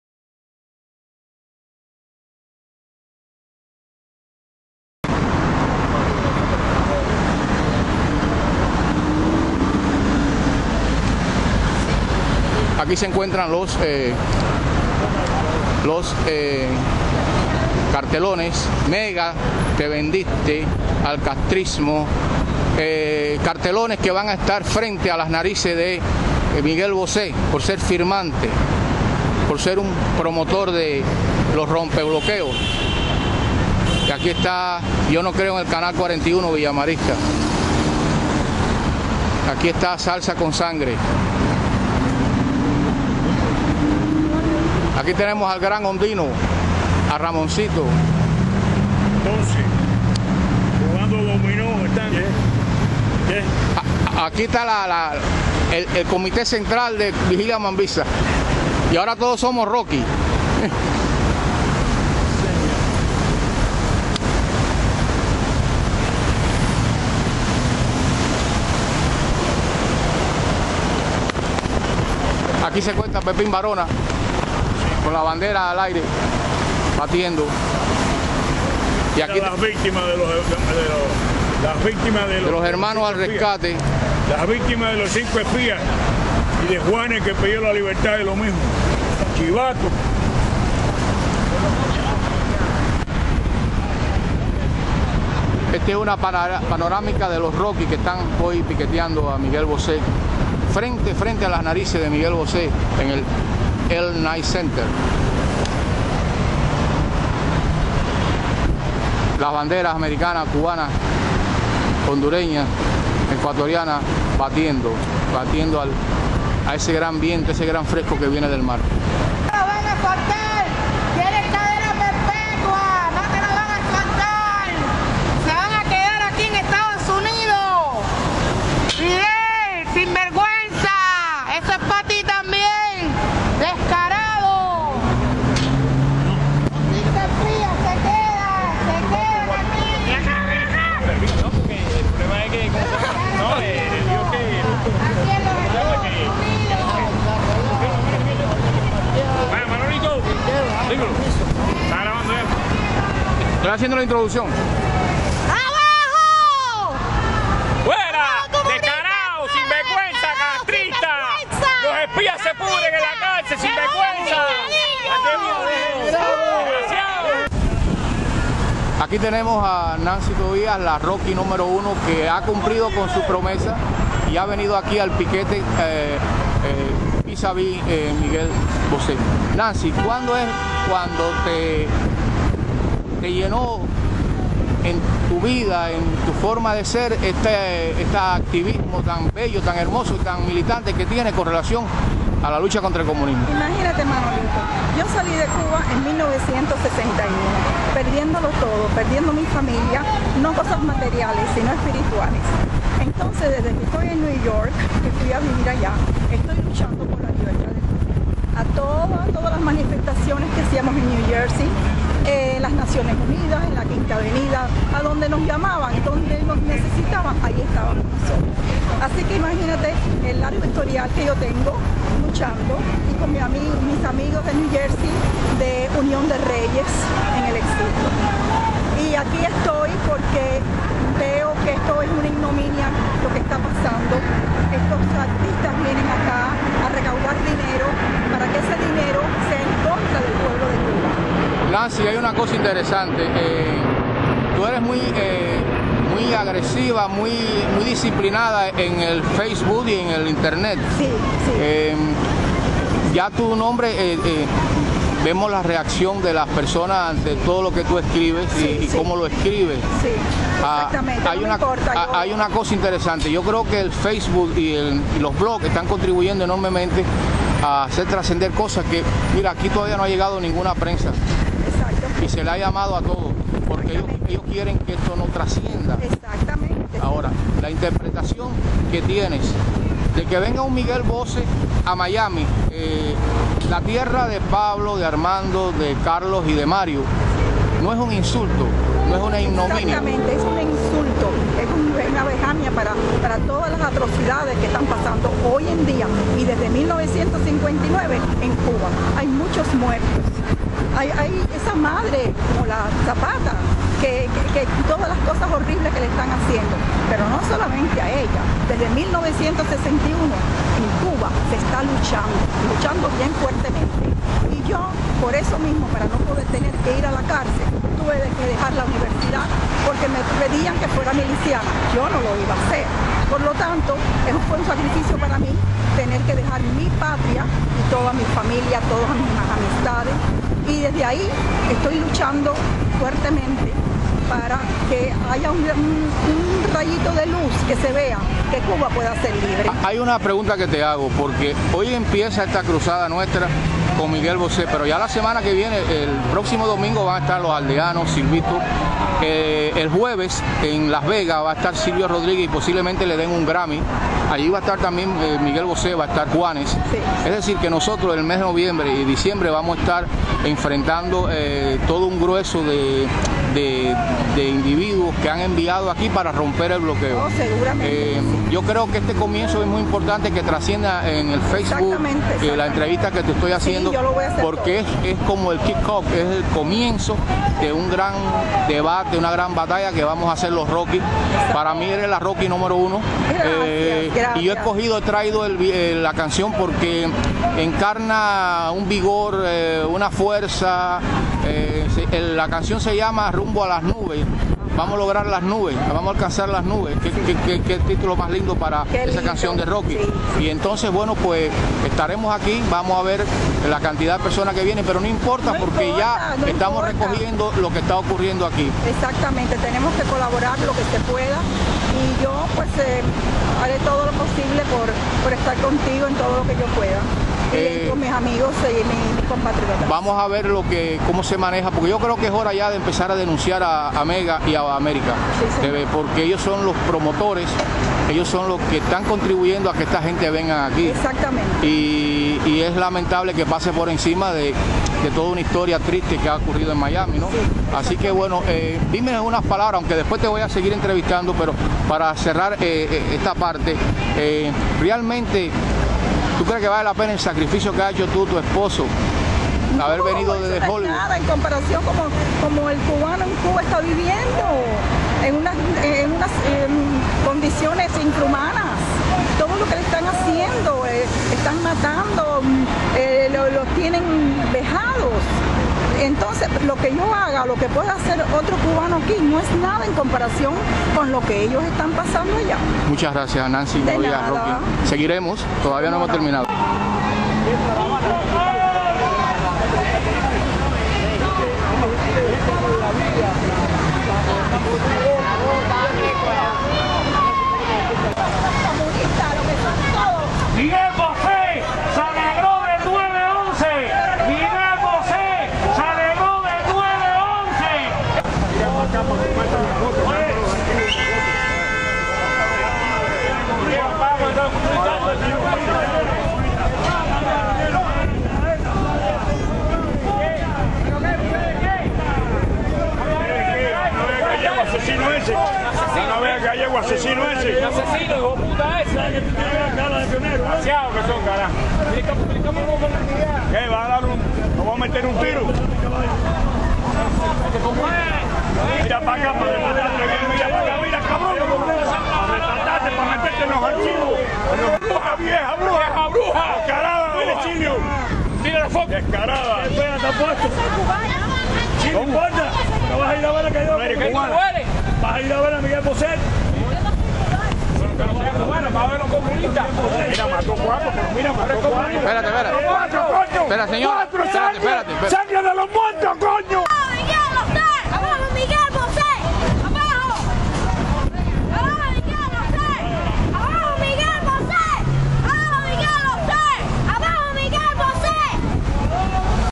Villagranadillo. Part II - Miguel Bose es piqueteado por "Los Rockys" en Miami.
Los "Rompe Bloqueo" : Mega- America Teve- Aldeanos-Silvito, Candido Fabre.